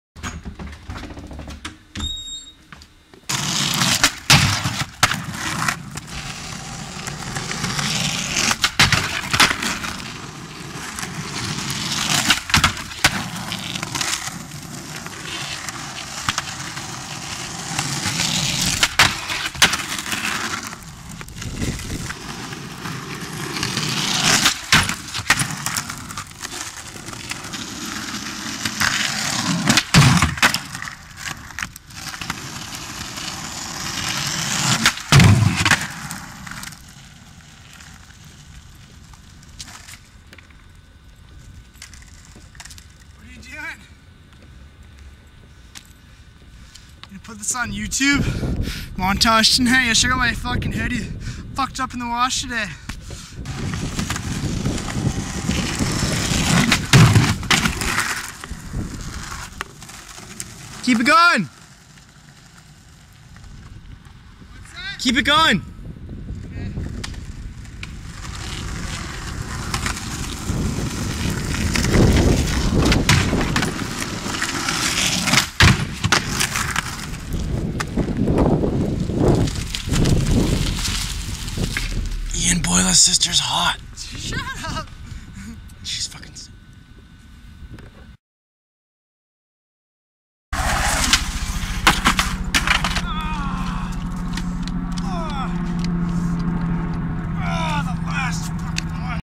Cold windy day, new "Optex" led light !
skated with ian boila that day, his sisters pretty hot. had fun and filmed a few tricks jus fuckin around. oh yea, new video light :)